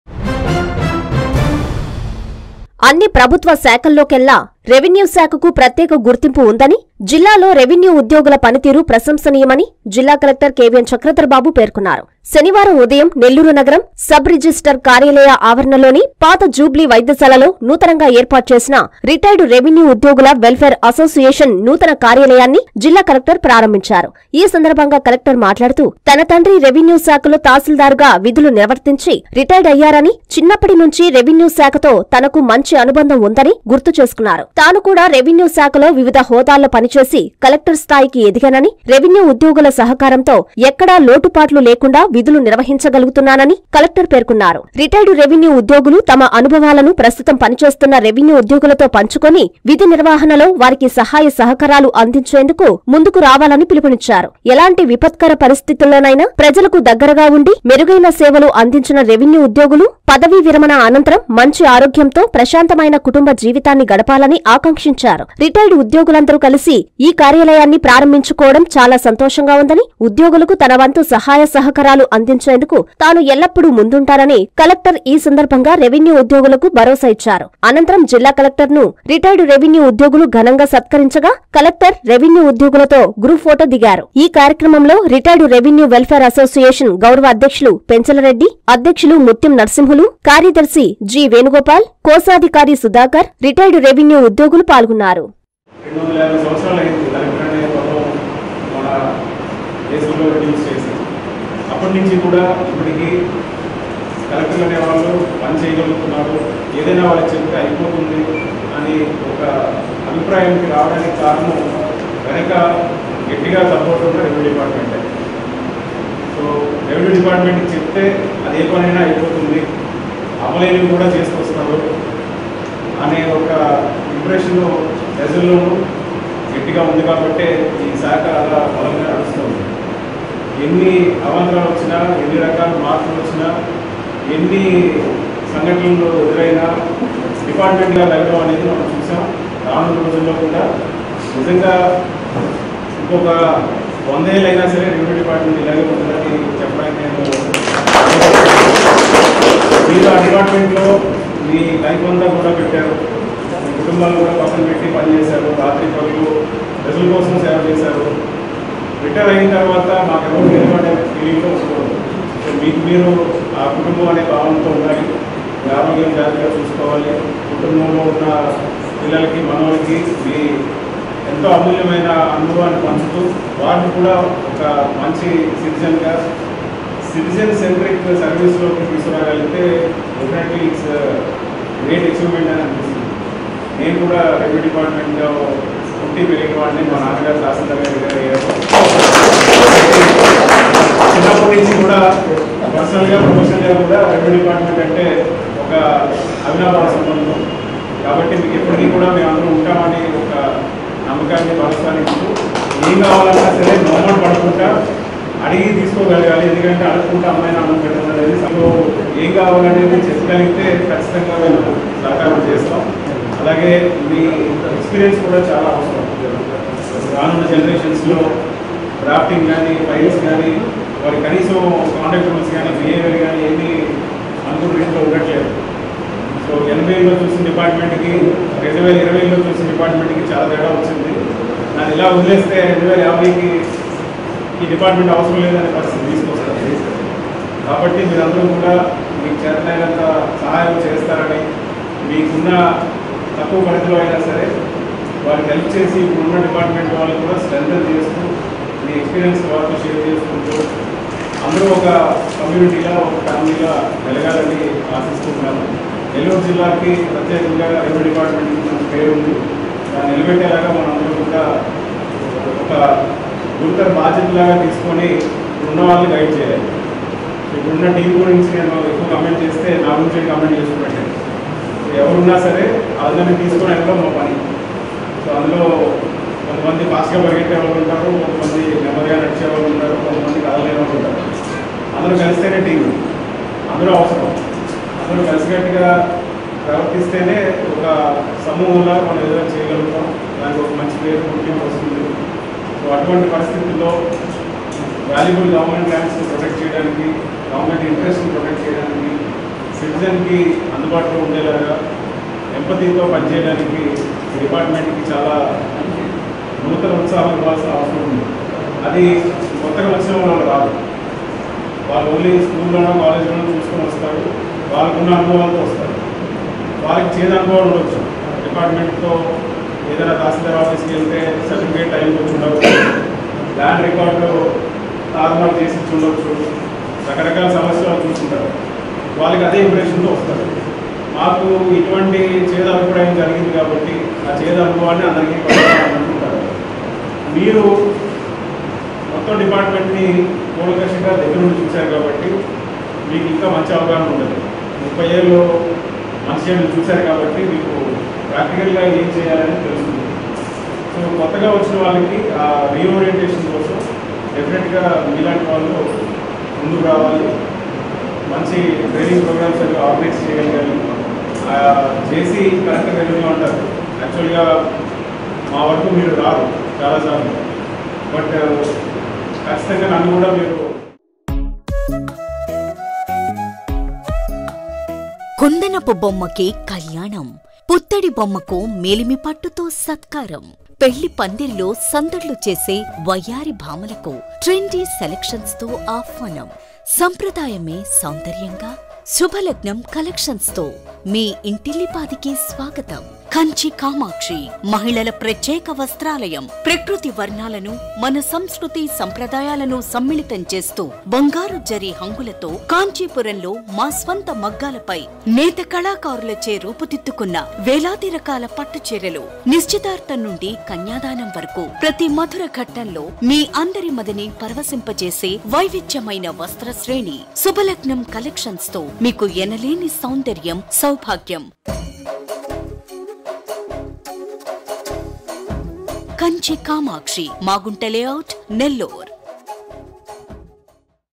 మీకు ఎల్ల‌ప్పుడూ అండ‌గా ఉంటా...
- రెవెన్యూ ఉద్యోగుల‌కు భ‌రోసా
- రిటైర్డ్ రెవెన్యూ ఉద్యోగుల వెల్ఫేర్ అసోసియేషన్ నూతన కార్యాలయాన్ని జిల్లా కలెక్టర్